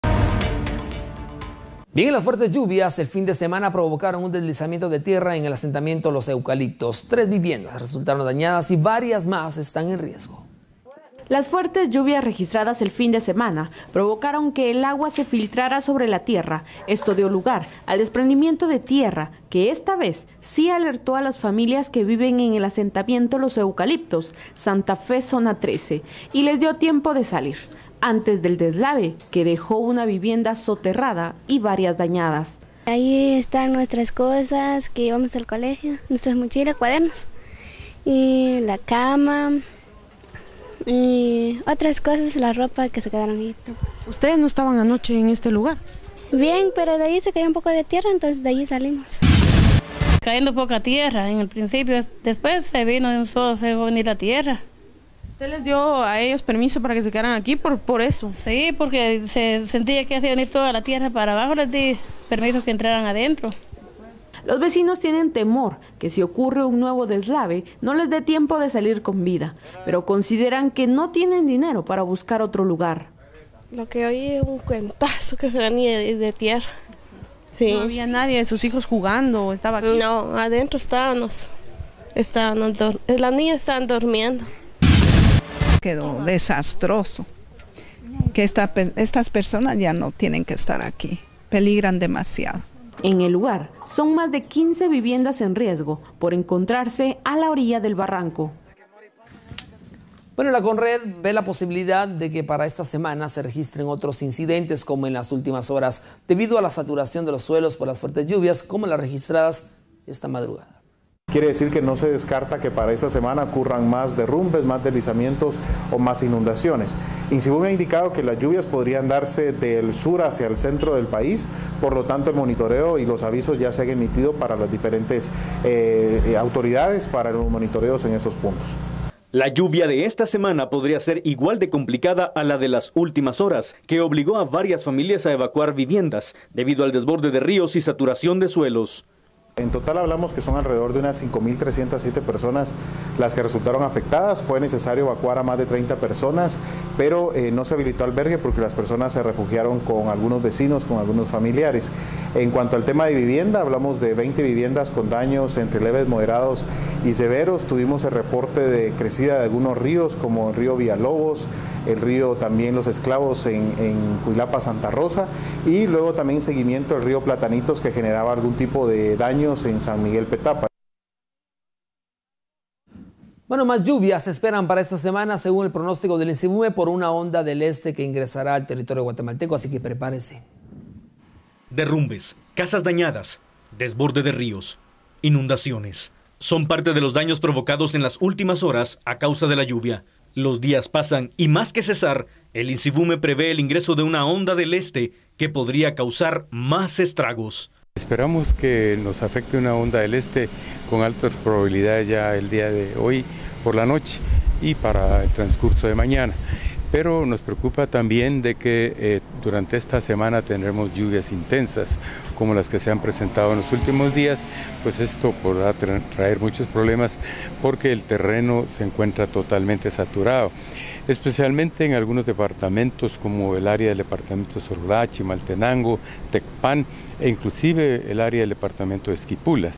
LLuvias provocan deslizamiento en asentamiento Los Eucaliptus
Tres viviendas resultaron dañadas por el alud